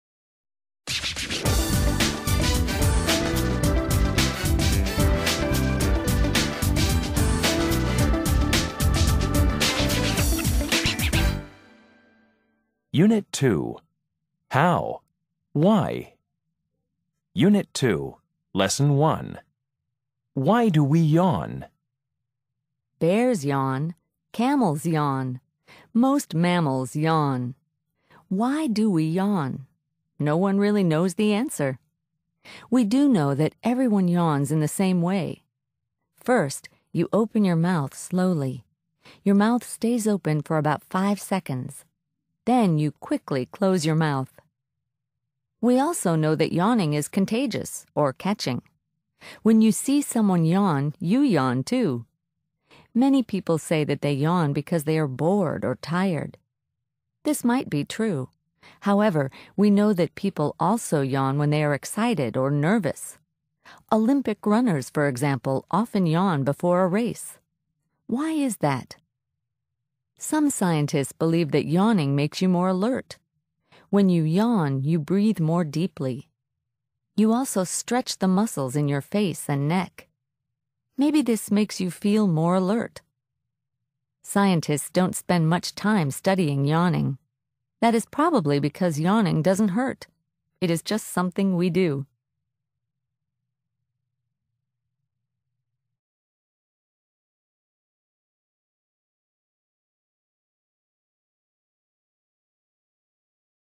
Facts and Figures Unit 2 Lesson 1 Why Do We Yawn?
Why Do We Yawn?

Bears yawn. Camel yawn. Most mammals yawn. Why do we yawn? No one really knows the answers.
 We do know that everyone yawns in the same way. First you open your mouth slowly. Your mouth stays open for about five seconds. Then you quickly close your mouth.
 We also know that yawning is contagious, or catching. When you see someone yawn, you yawn, too. Many people say that they yawn because they are bored or tired. This might be true. However, we know that people also yawn when they are excited for nervous. Olympic runners, for example, often yawn before a race. Why is that?
 Some scientists believe that yawning makes you more alert. When you yawn, you breathe more deeply. You also stretch the muscles in your face and neck. Maybe this make you feel more alert.
 Scientists don’t spend much time studying yawning. That is probably because yawning doesn’t hurt. It is just something we do.